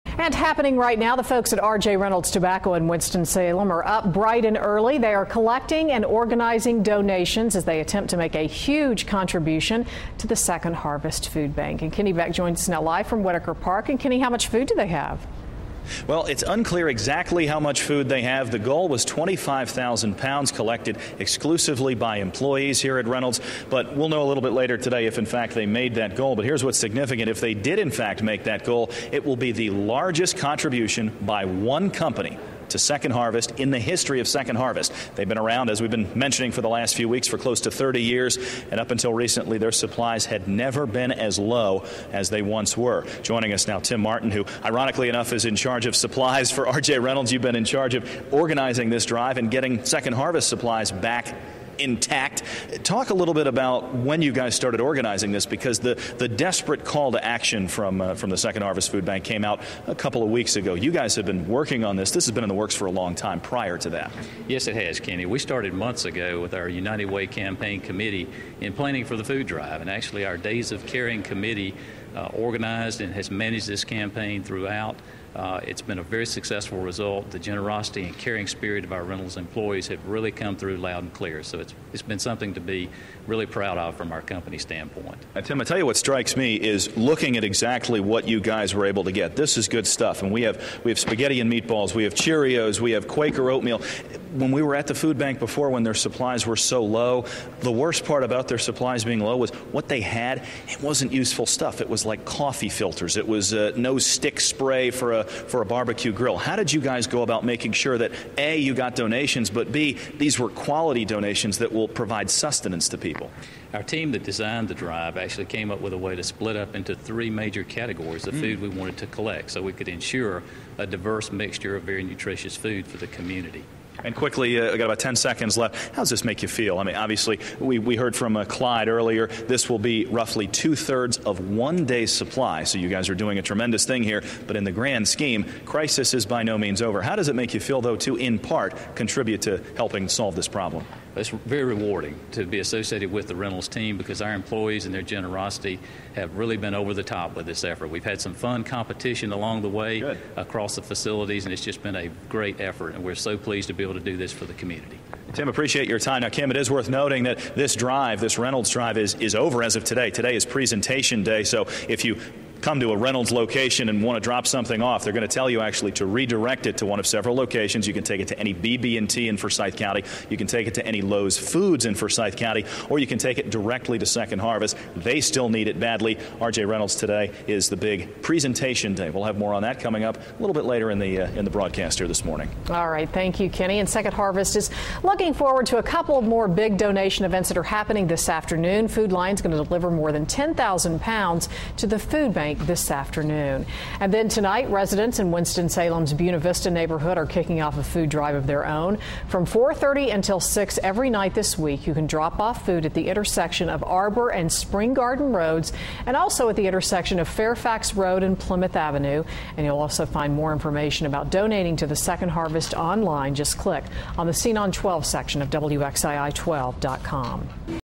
RJR Food Drive
RJ Reynolds needs your food donations to help stock the Second Harvest Food Bank...